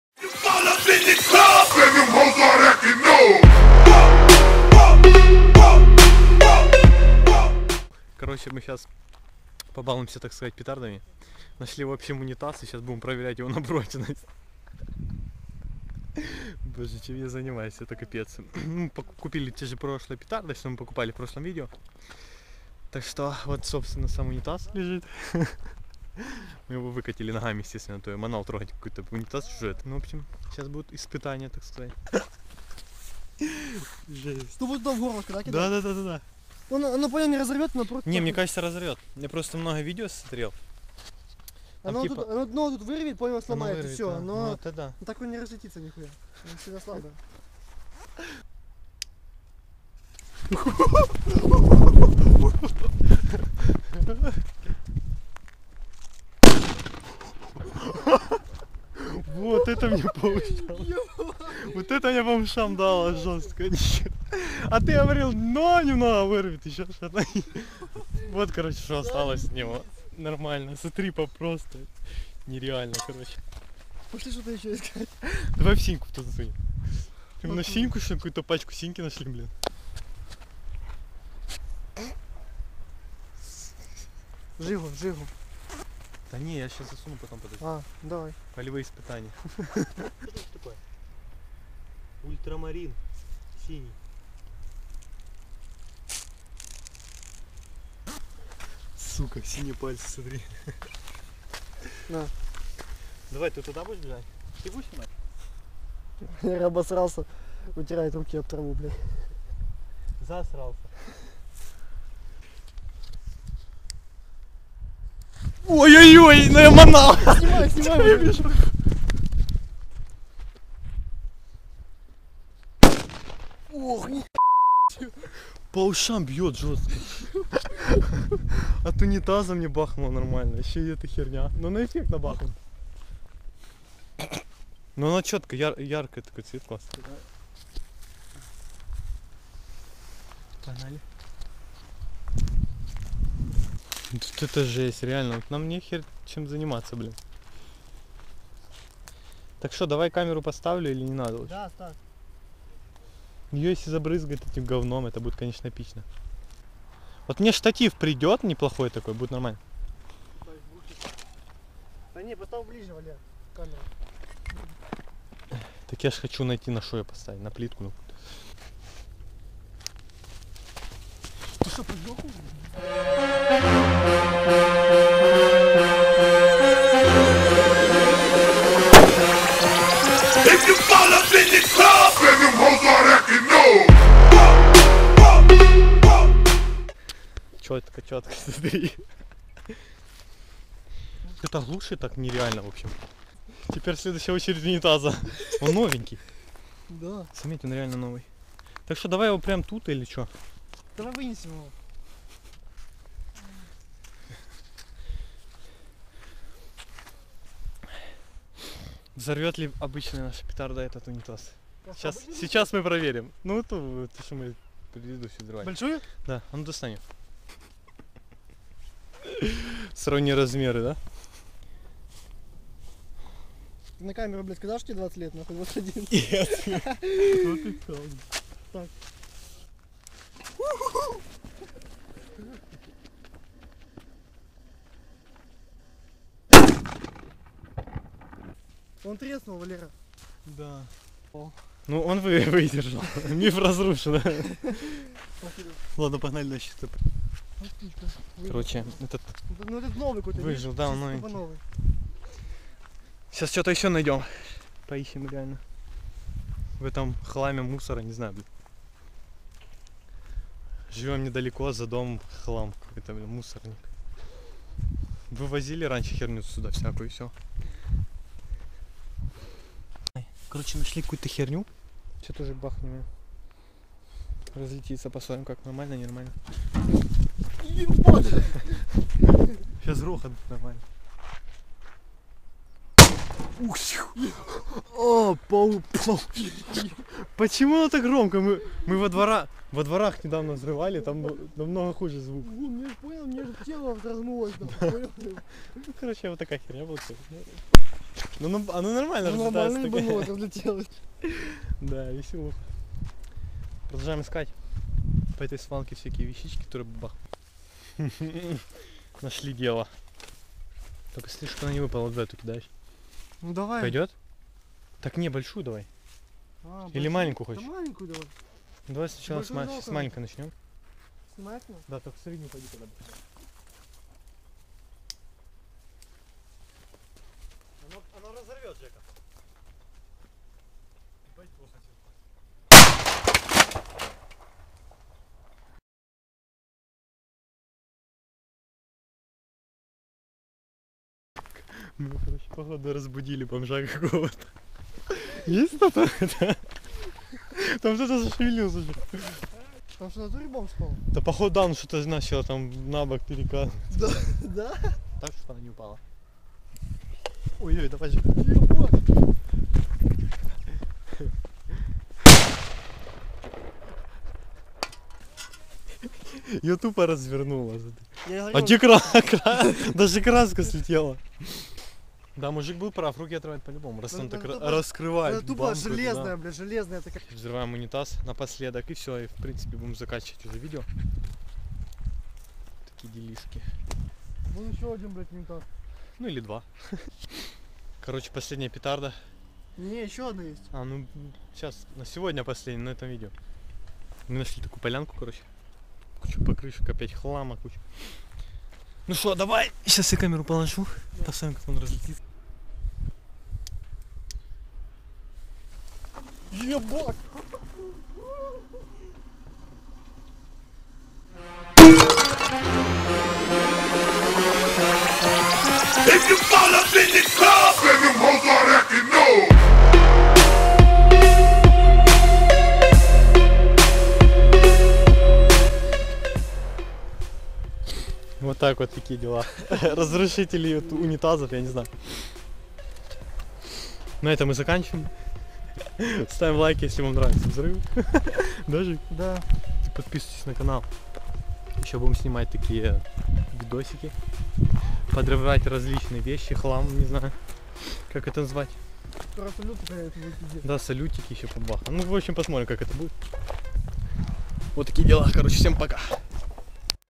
Взрывной хлам: мощная бомбочка
Никогда не повторяйте увиденное!

Новая рубрика Взрывной хлам. Тестируем петарды и взрываем все что только можно. Смотрите,коментируйте и оценивайте!